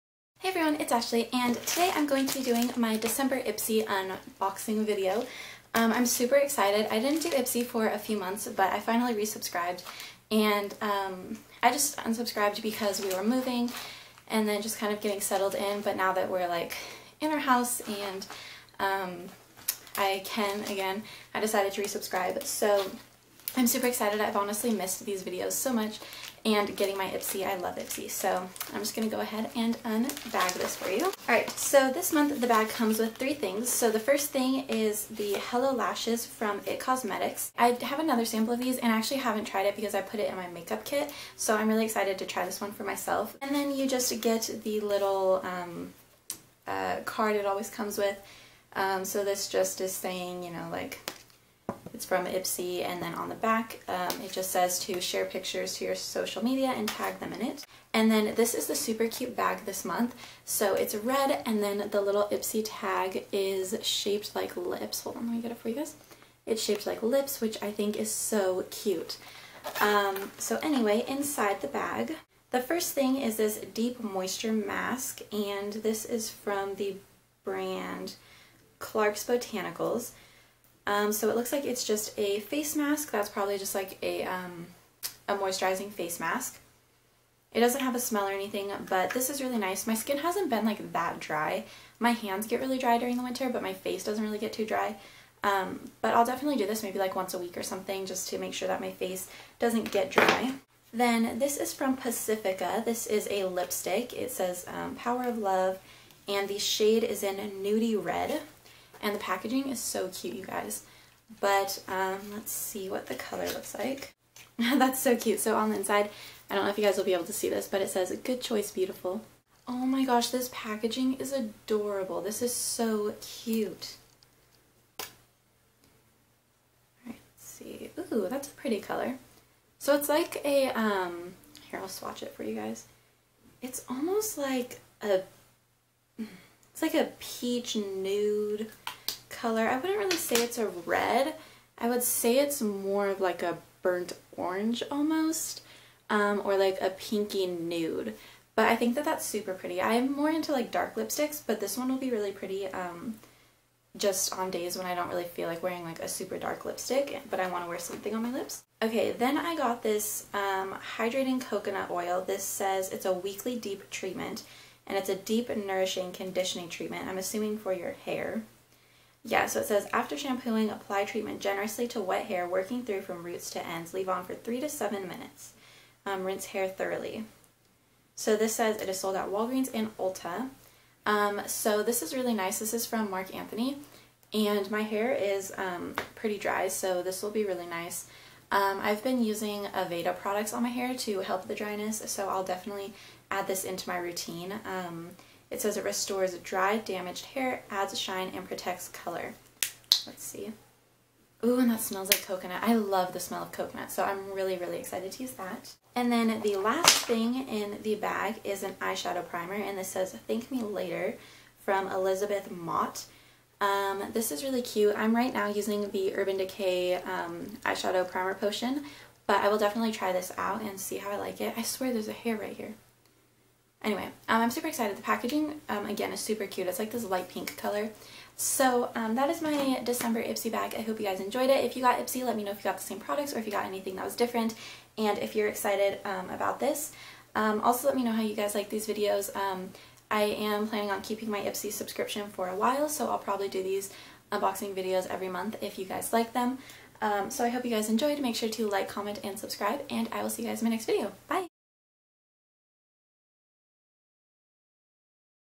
Ipsy Unboxing December 2015!
Hey everyone! I love monthly boxes, I think they are so much fun so I resubscribed to Ipsy! Let me know if you want me to keep doing unboxing videos!